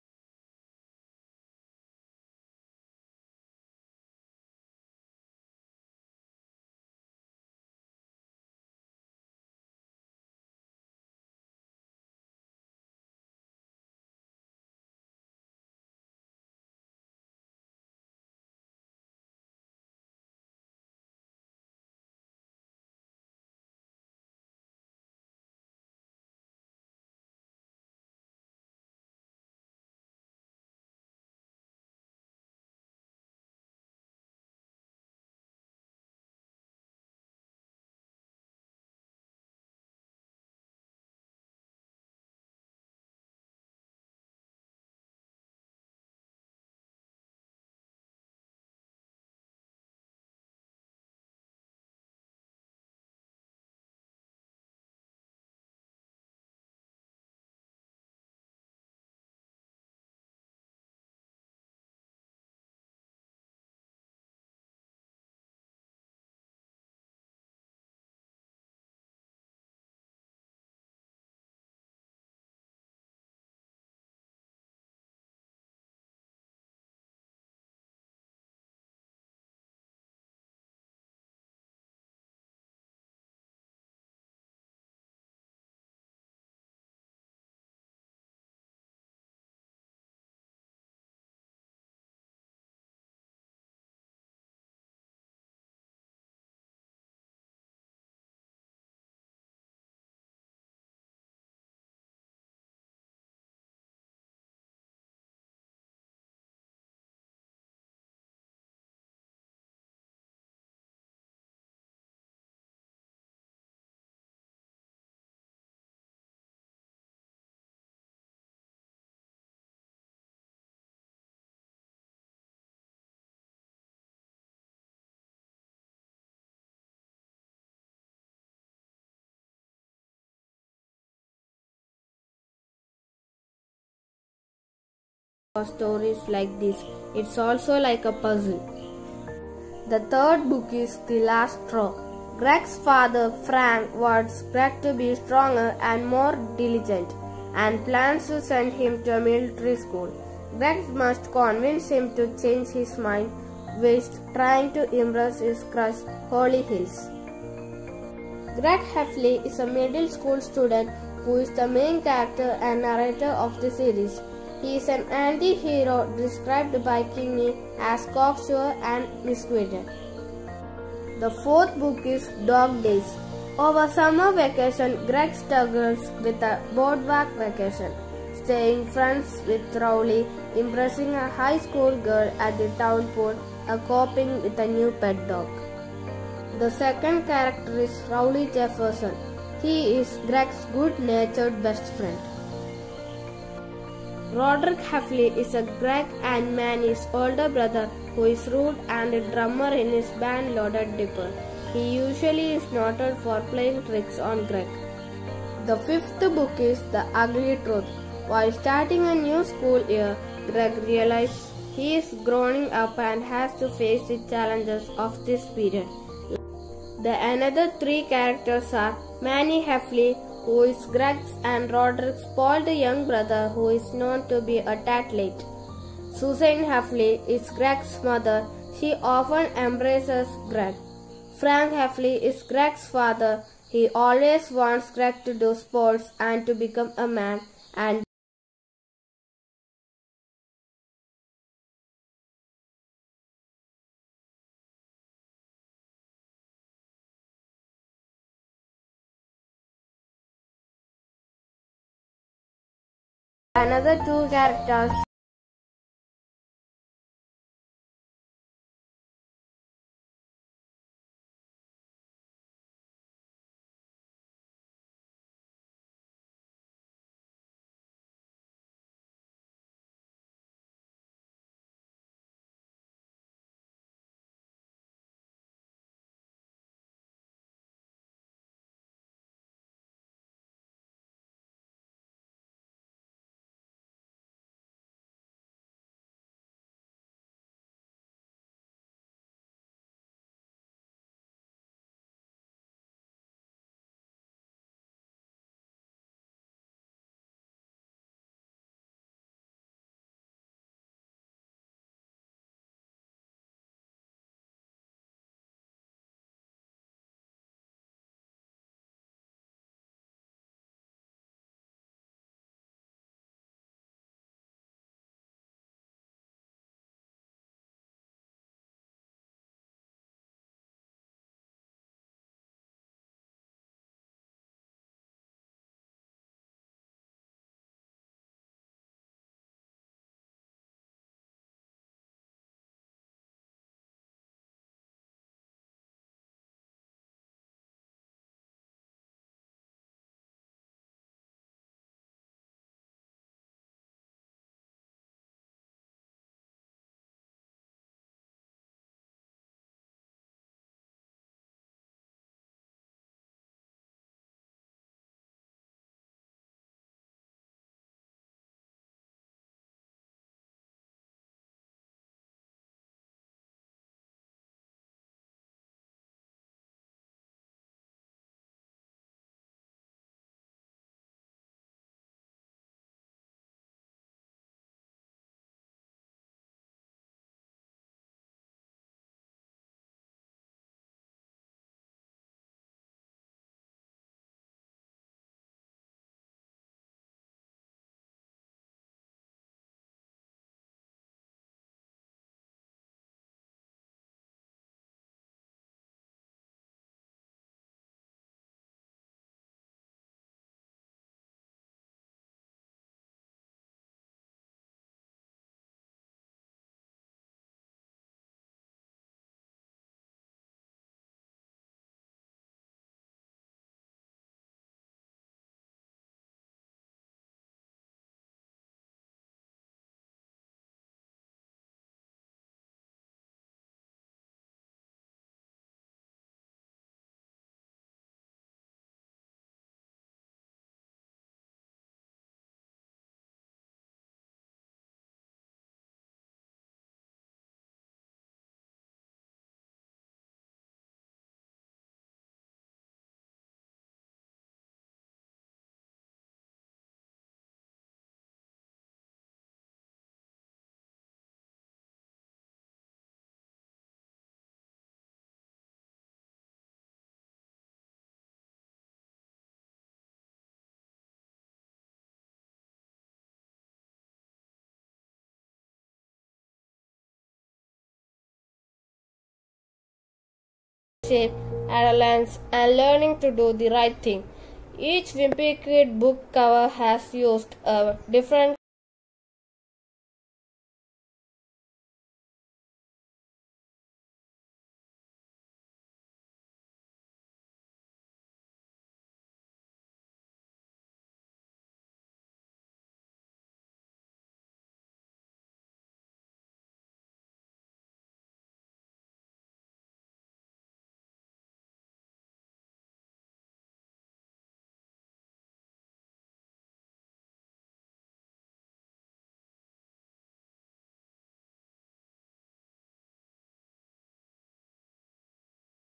An American Author and Cartoonist Jeff Kinney | creator of the children book
@rainishvlog29 An American Author and Cartoonist Jeff Kinney | creator of the children book

Diary of a Wimpy Kid | Best selling author Jeff Kinney
Diary of a Wimpy Kid is an American children's book series and media franchise created by American author and cartoonist Jeff Kinney. The series follows Greg Heffley, a middle-schooler who illustrates his daily life in a diary. Kinney spent eight years working on the first book before showing it to a publisher.